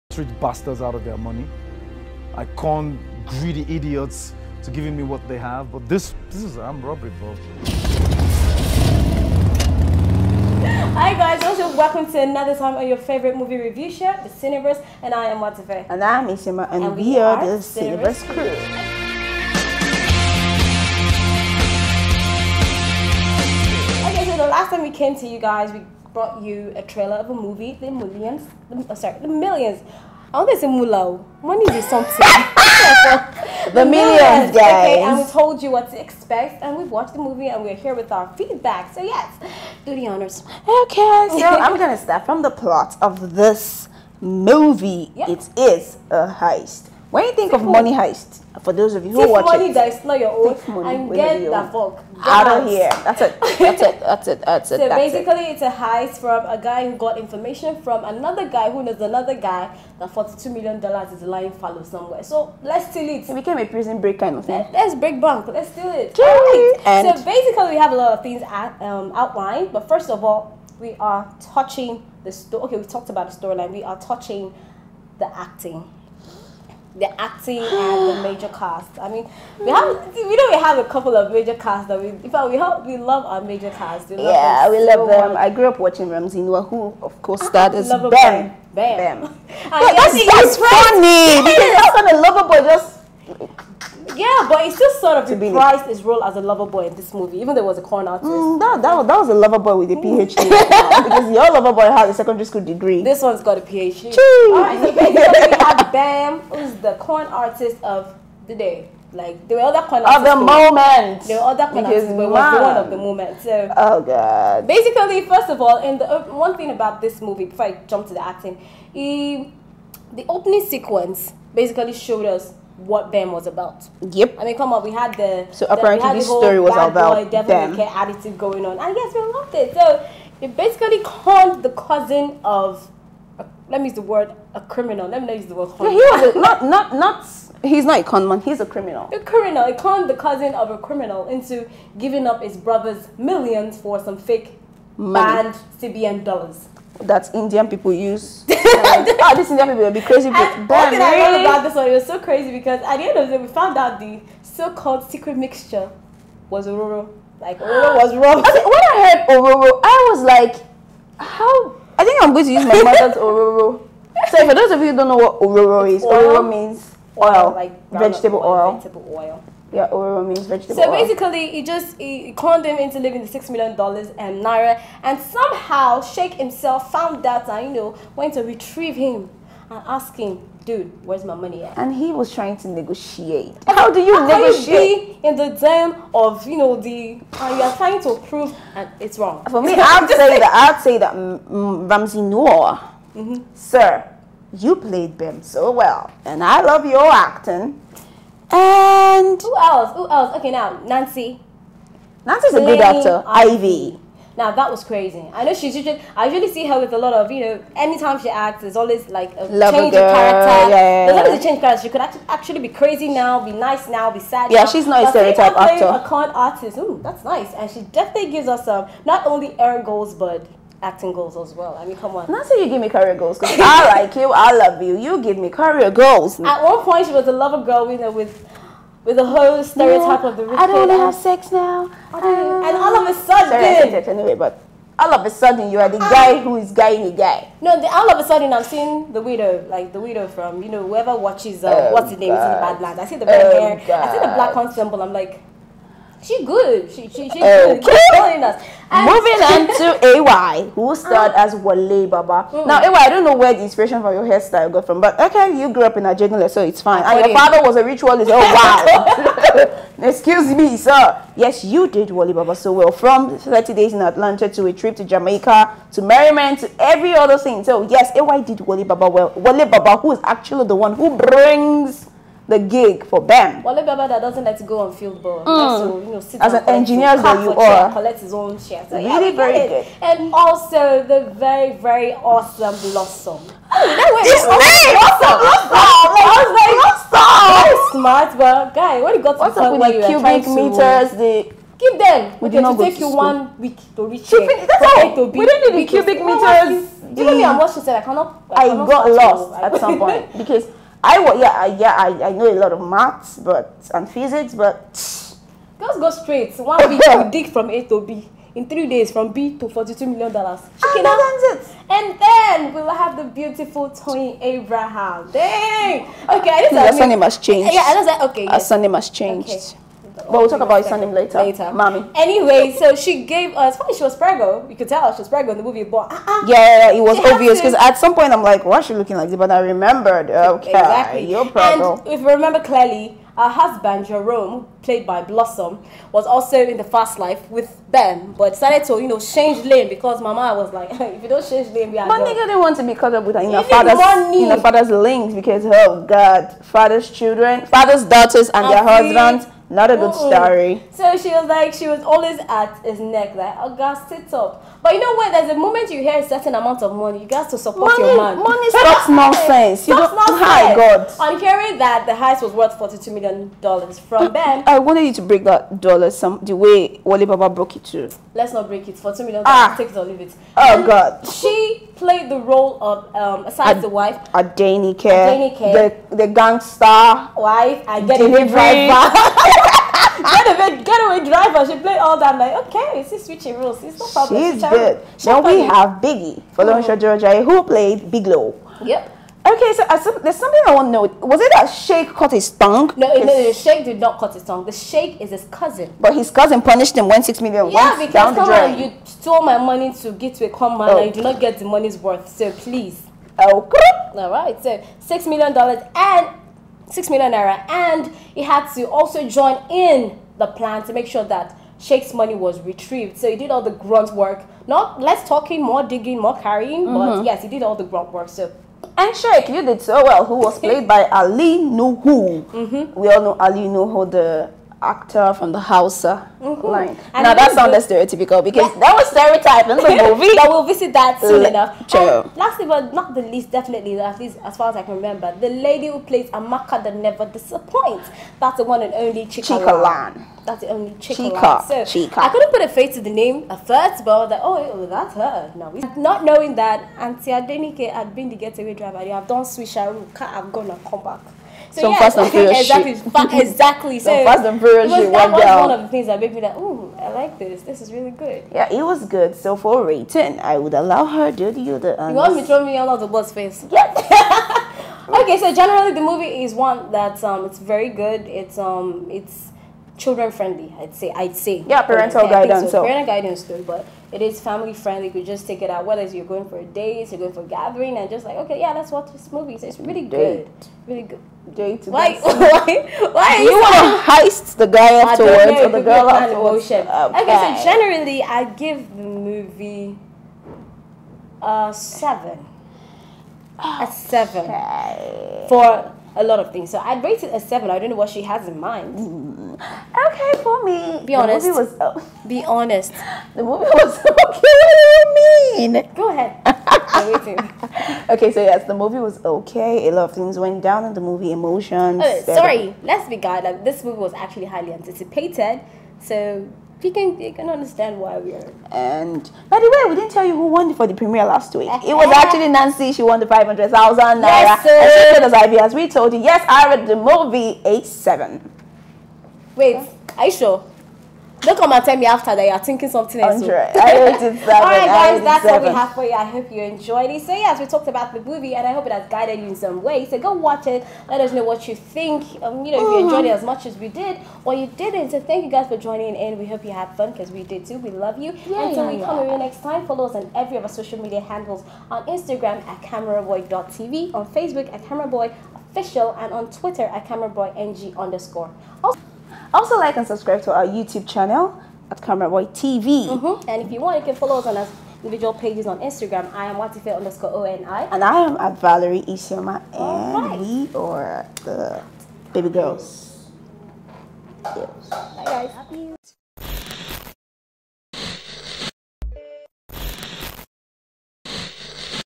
The Millions (Nollywood Movie) - Full Review
The Millions, produced by Chika Lann; directed by Toka Mcbaror and Tunde Apalowo was a feel-good Nollywood thriller! It was an audience pleaser. It is supposed to be of the crime genre, but our host humbly see it in a different light.

A Con artist is known to sap money out of unsuspecting greedy men. He builds a team of money-hungry men to steal a huge sum of money to the tune of millions. Planning this operation and the actual execution of it reveal what each one of them truly wants.

In this video the Cineverse hosts gave their honest opinion on what they think about the movie.